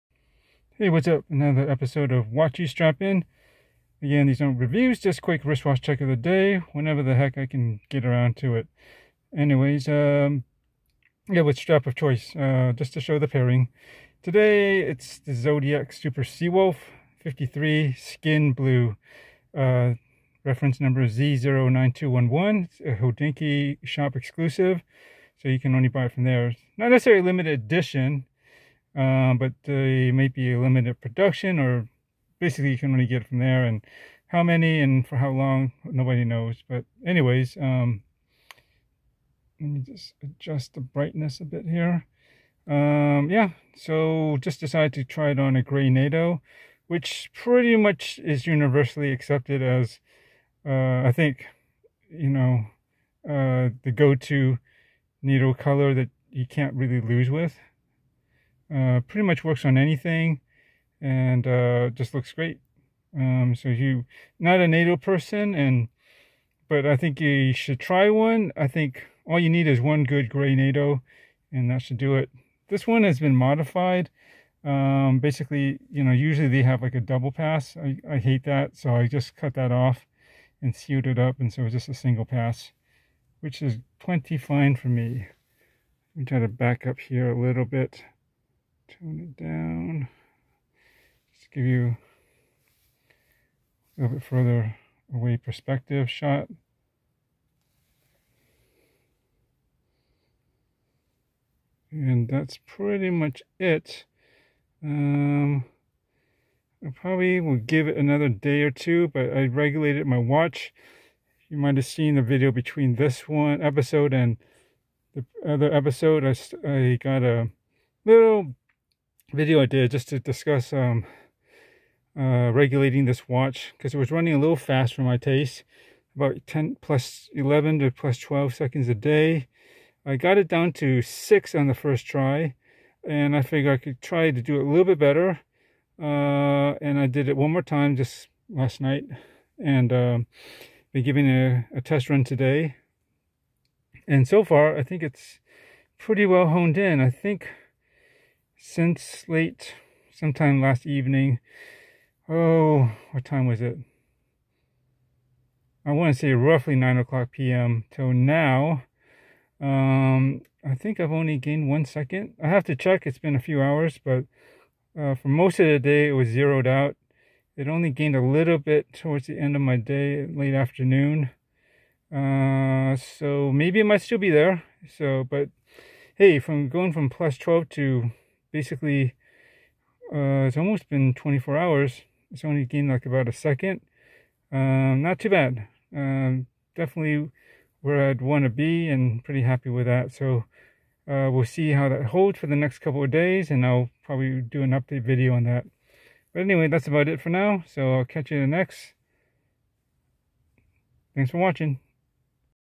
Watch U Strappin'?! Ep. 219 - Zodiac Super Sea Wolf 53 Skin 'Blue' ZO9211
A grey NATO can look good on just about any watch. Even if you're not a NATO strap guy or gal, it's still good to have at least one to try once in a while. And, if you're going to get just one, get it in grey.

My Super Sea Wolf Saturday sporting the Skin 'Blue' ZO9211